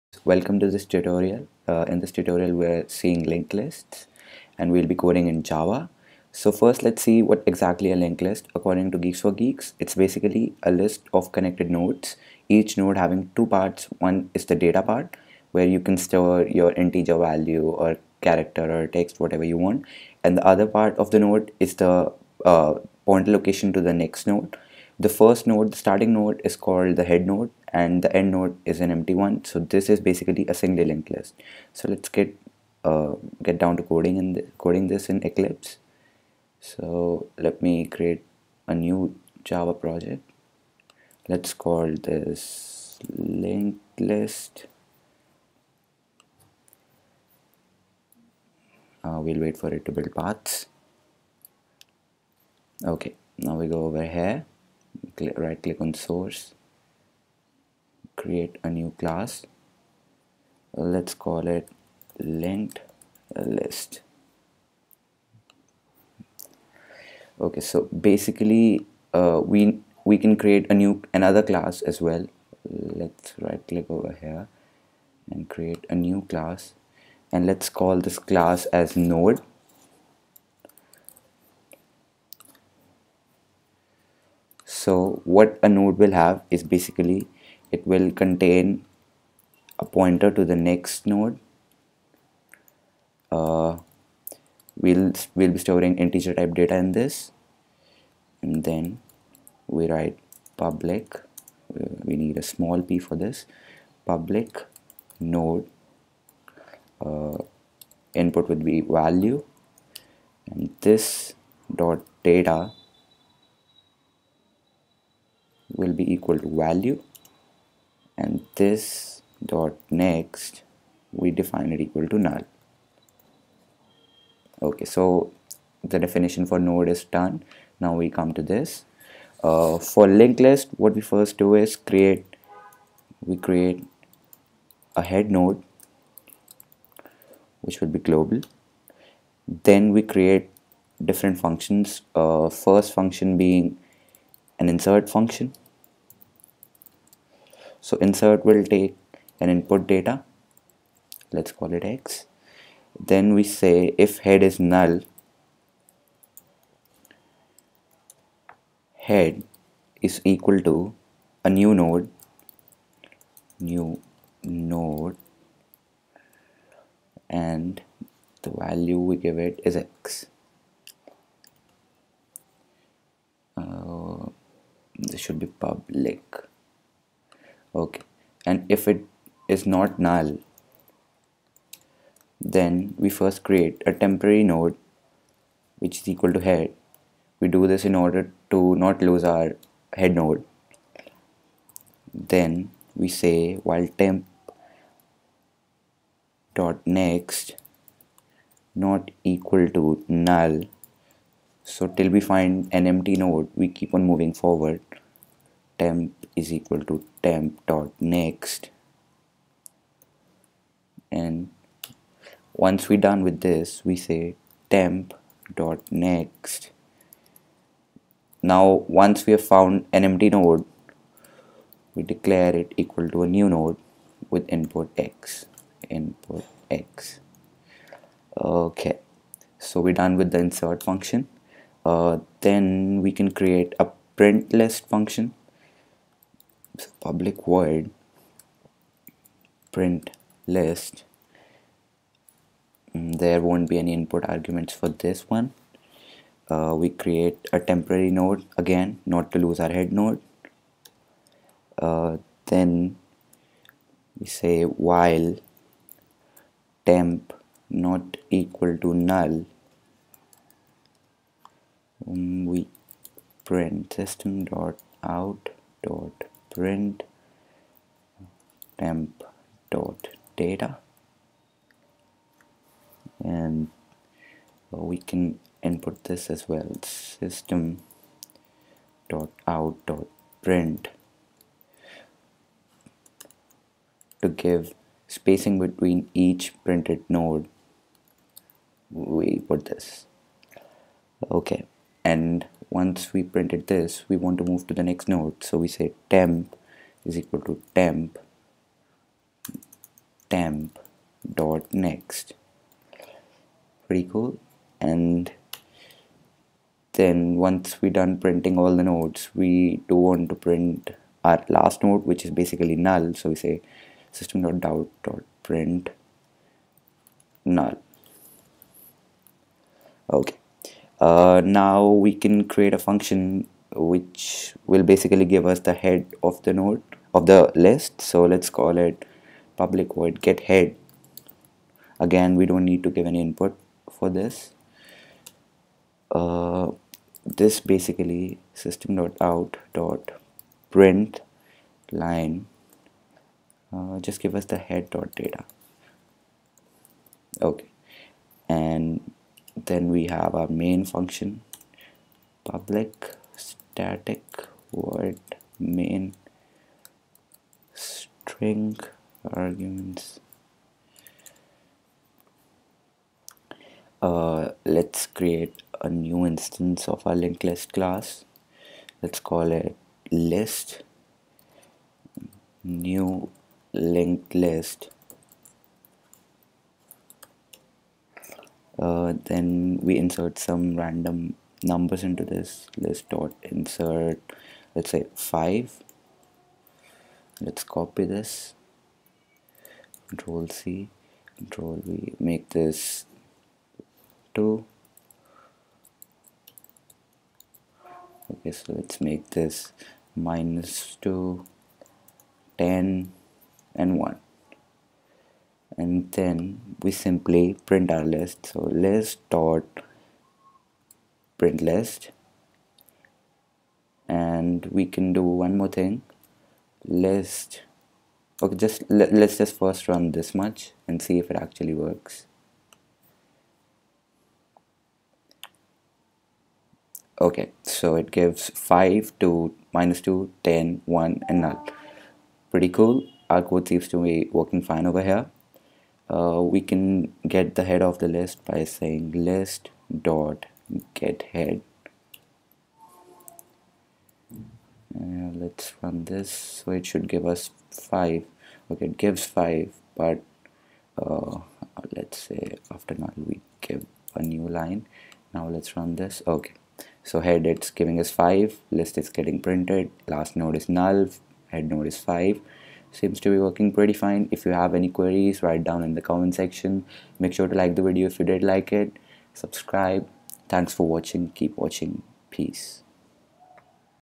Linked List Implementation | Code in Java
This video is a step by step tutorial on how to program Linked List data structure in Java using Eclipse. Source Code